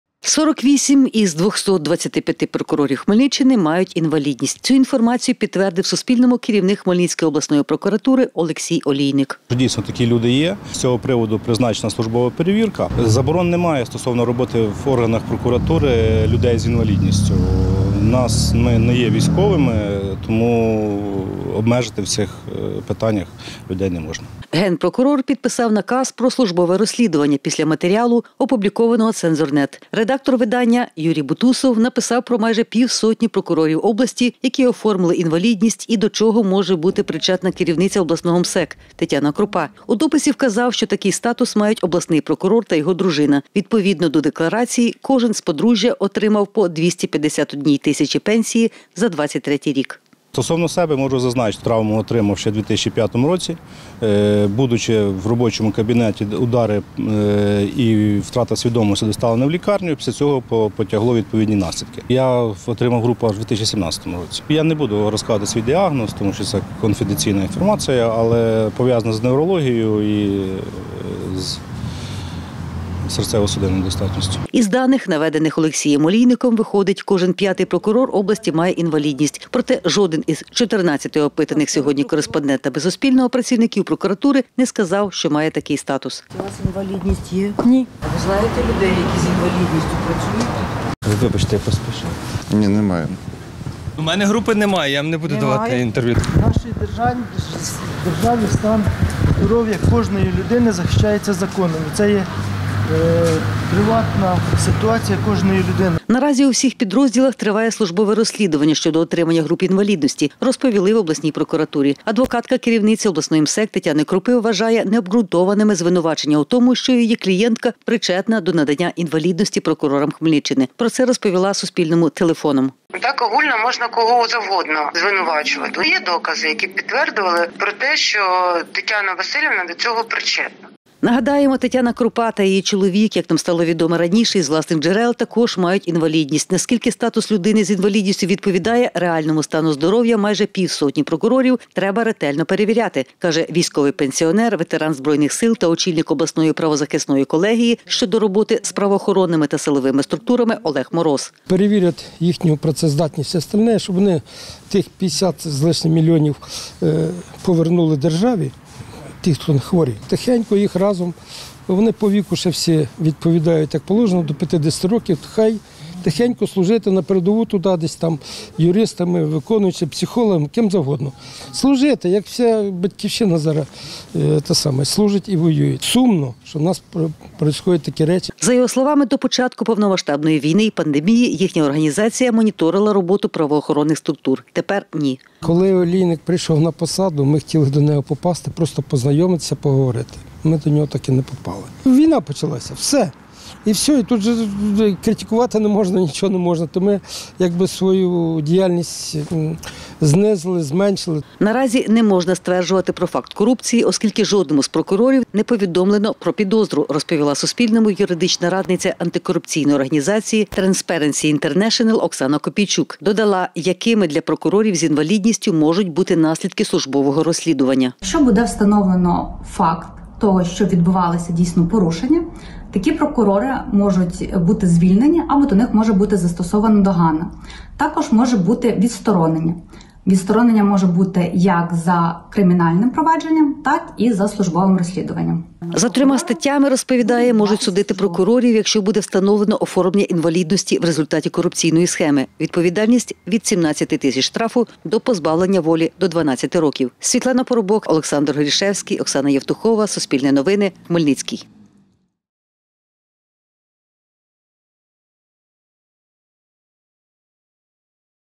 Якими можуть бути наслідки службового розслідування для прокурорів з інвалідністю
Кожен п'ятий прокурор Хмельниччини має інвалідність. Керівник обласної прокуратури Олексій Олійник сьогодні, 17 жовтня підтвердив Суспільному, що серед підлеглих і у нього самого такий статус є.  Доки матеріал готувався, офіс Генпрокурора повідомив про його звільнення з посади за власним бажанням.